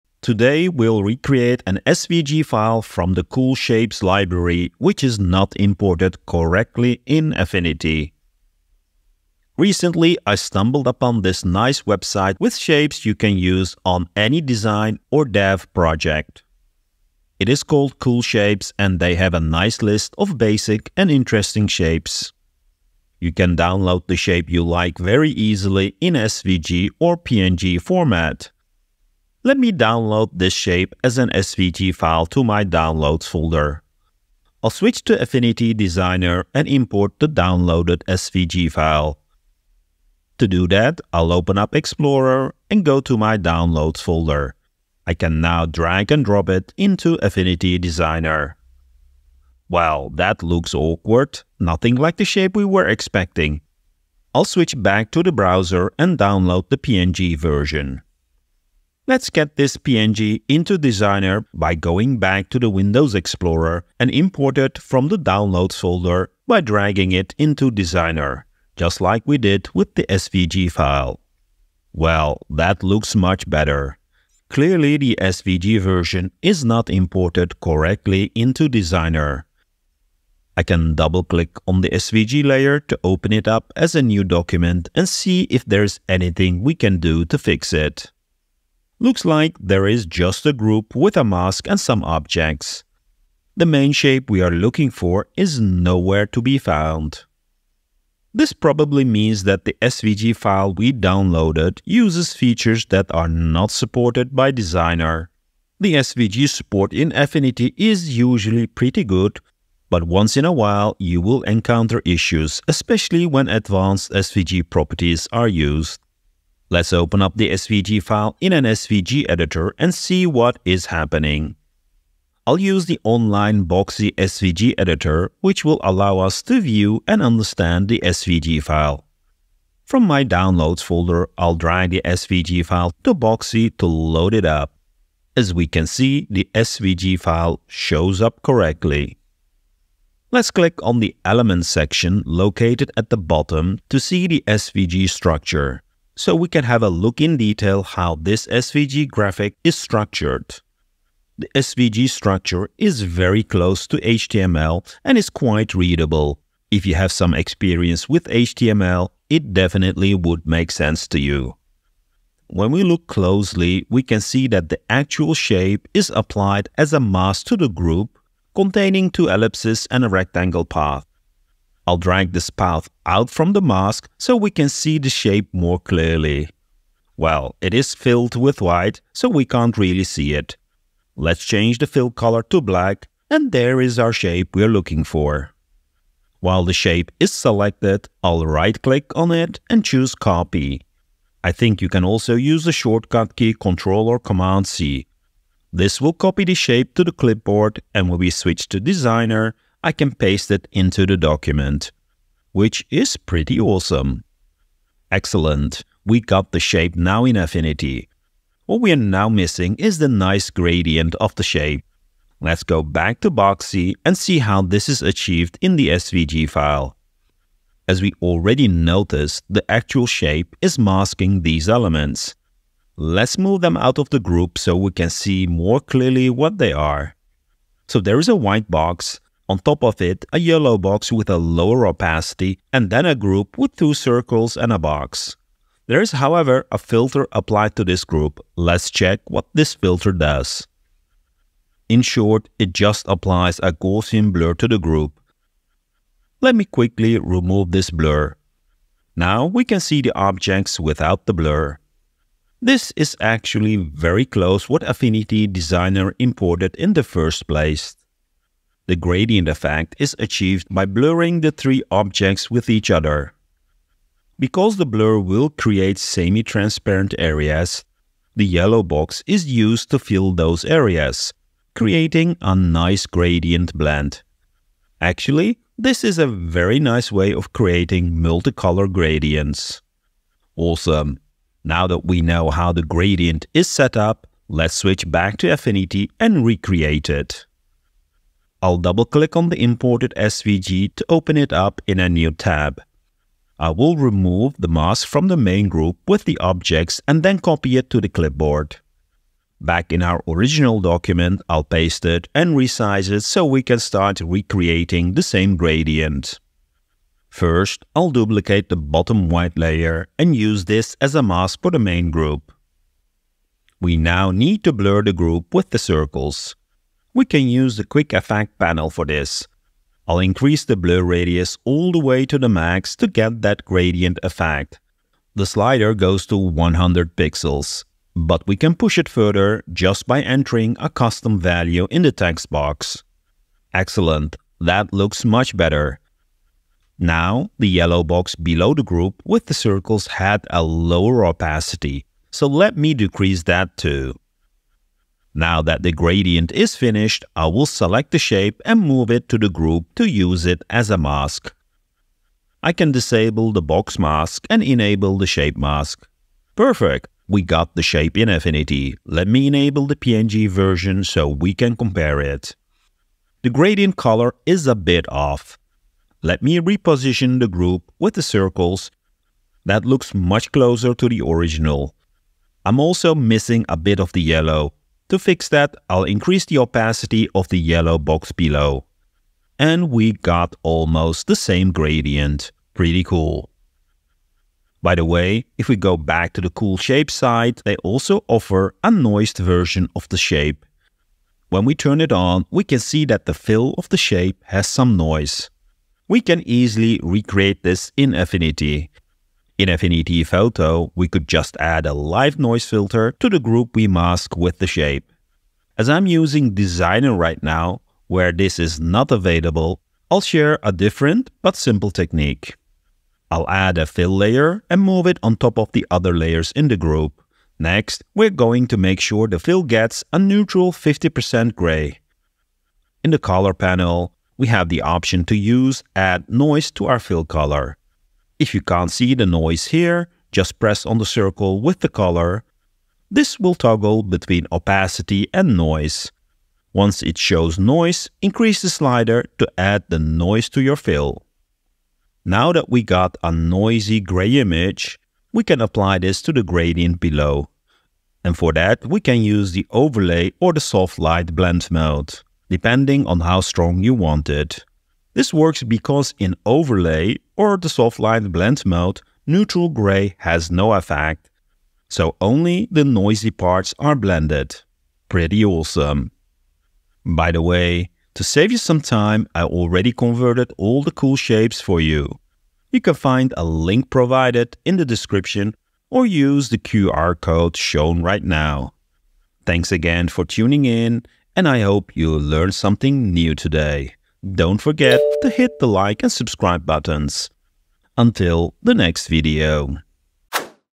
Importing SVG issues in Affinity Designer or Photo? Let's learn how to recreate the SVG in Affinity
Sometime importing from SVG fails in Affinity. In this video we will take a quick look how we can use Boxy app to analyze an SVG file and use that information to recreate the same SVG graphic in Affinity.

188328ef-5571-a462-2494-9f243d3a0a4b